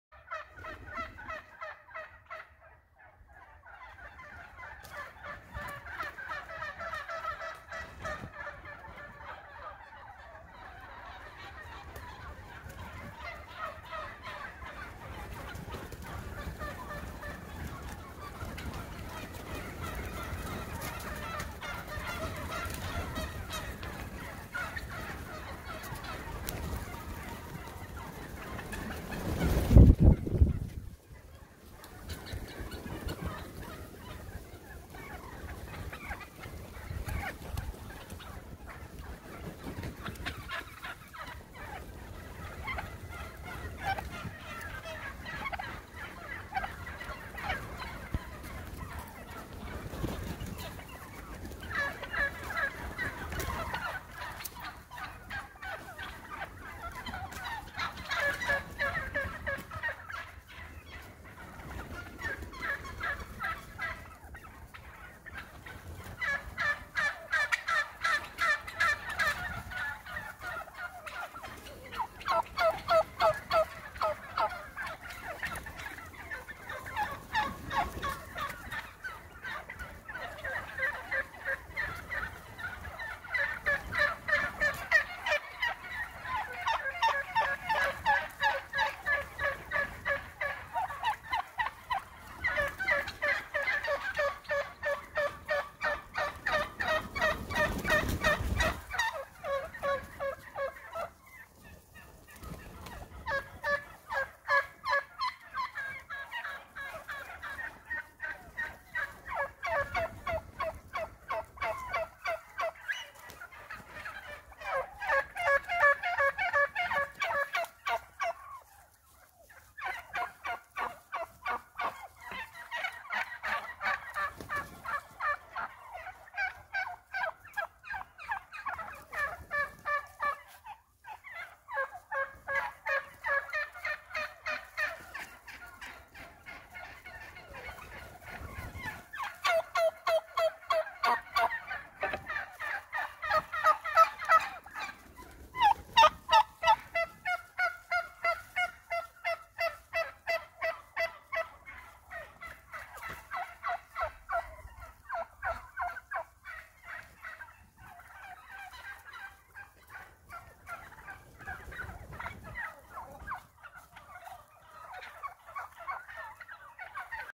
How many turkeys can you count?
Another typical roll out of bed for the wild turkeys that roost on our property.